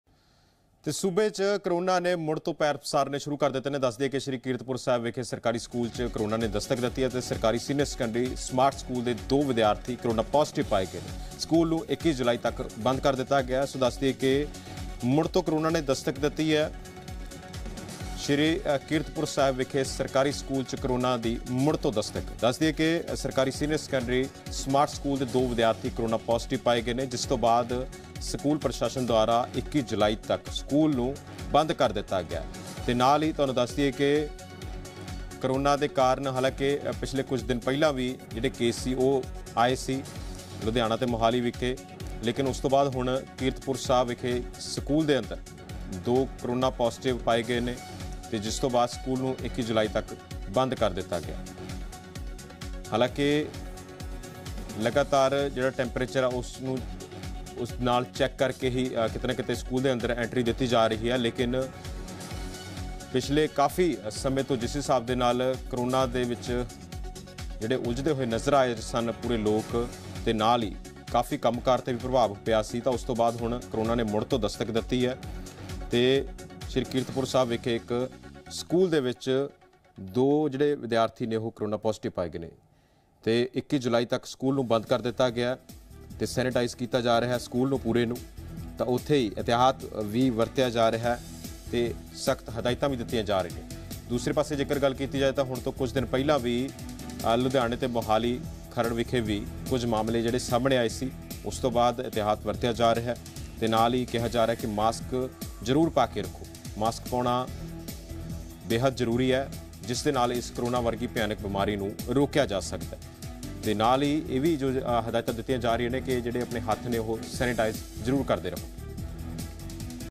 Punjab News: ਸ੍ਰੀ ਕੀਰਤਪੁਰ ਸਾਹਿਬ ਵਿਖੇ ਸਰਕਾਰੀ ਸਕੂਲ 'ਚ ਕੋਰੋਨਾ ਦੀ ਦਸਤਕ | ZeePHH
ਸ੍ਰੀ ਕੀਰਤਪੁਰ ਸਾਹਿਬ ਵਿਖੇ ਸਰਕਾਰੀ ਸਕੂਲ 'ਚ ਕੋਰੋਨਾ ਦੀ ਦਸਤਕ